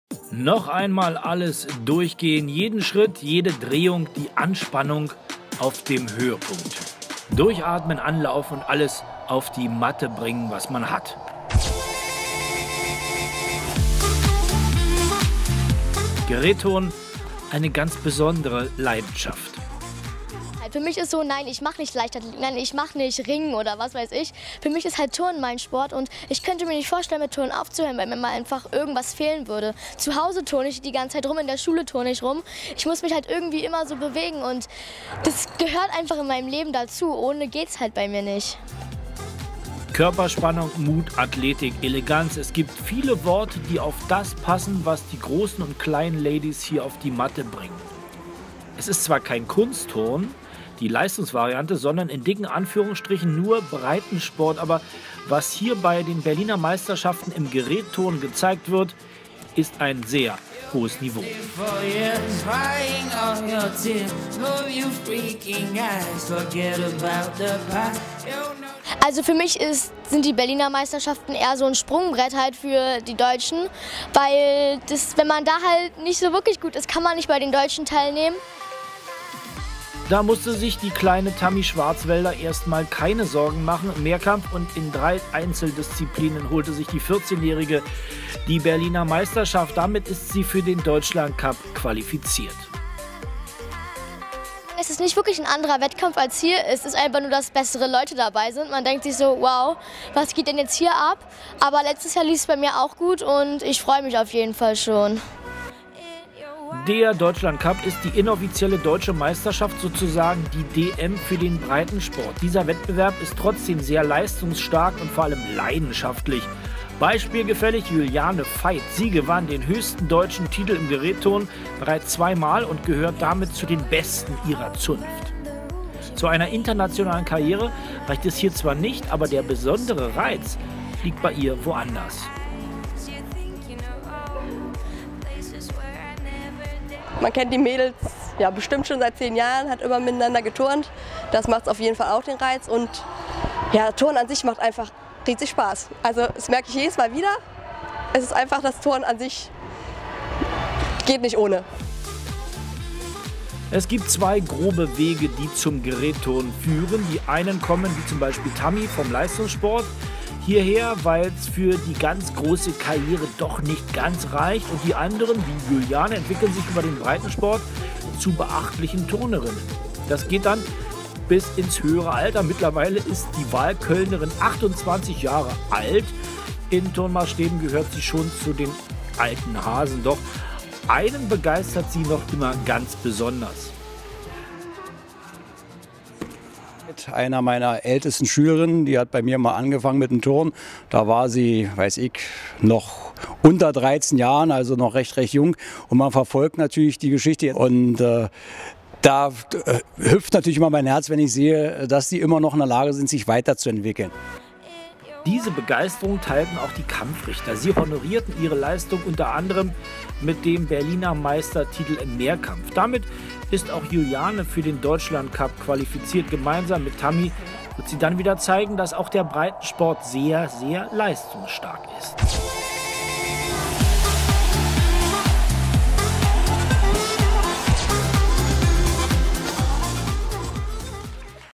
Berliner Meisterschaften 2015 im Gerätturnen
Körperspannung, Mut, Athletik, Eleganz. Es gibt viele Worte, die auf das passen, was die großen und kleinen Ladys hier auf die Matte bringen. Eins sind sie auf jeden Fall, das beste was die Berliner Gerätturnwelt zu bieten hat! Wir haben zwei der besten Mädels genau unter die Lupe genommen und zeigen, dass diese Breitensportart sehr Leistungsstark ist!